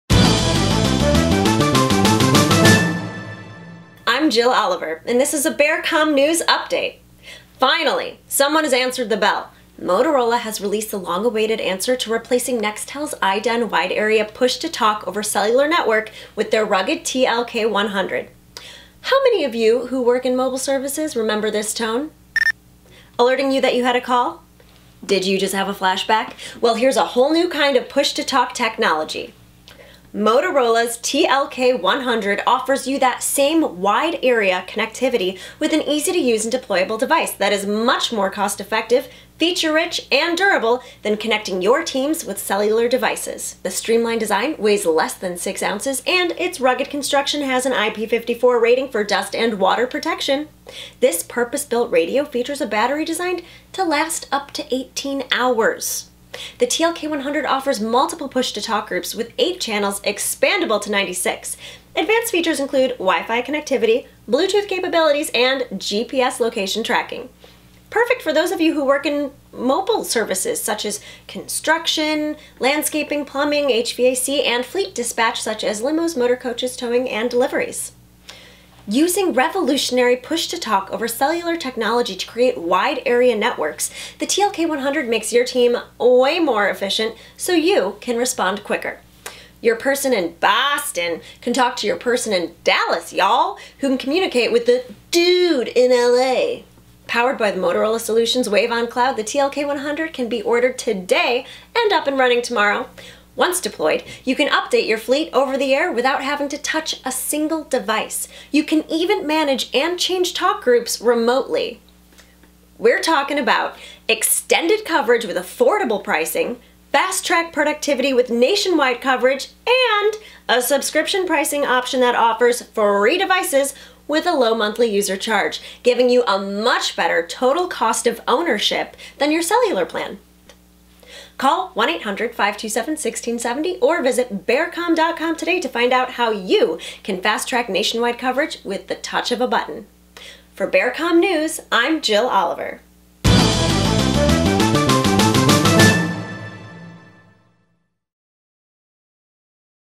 TLK 100: Two-Way Radio Communications over Nationwide Cellular Network
TLK 100 two-way radios from Motorola Solutions deliver Wide Area Network communications – even national coverage. Excellent for mobile service teams to boost connectivity, increase productivity, enhance service, and improve tracking. Find out how your mobile operations can benefit with the subscription-based TLK 100!